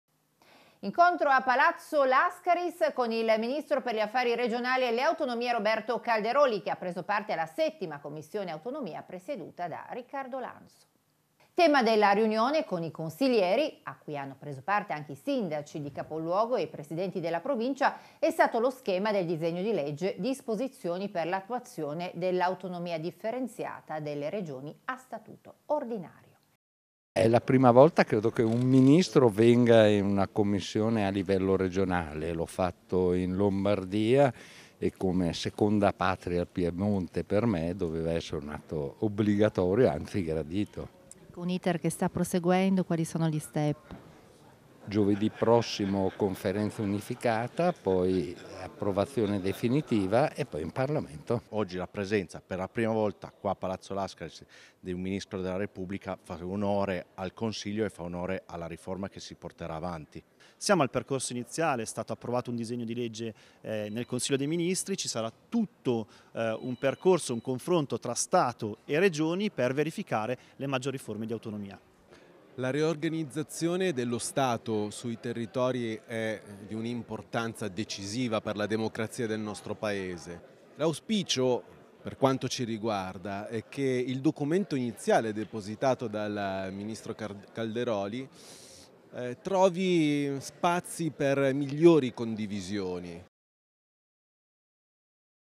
IL MINISTRO CALDEROLI IN COMMISSIONE AUTONOMIA
Il ministro per gli affari regionali e le autonomie Roberto Calderoli ha partecipato a un incontro in settima Commissione con numerosi consiglieri, sindaci e presidenti di Provincia. Tema della riunione è stato lo schema del disegno di legge “Disposizioni per l’attuazione dell’Autonomia differenziata delle Regioni a statuto ordinario”.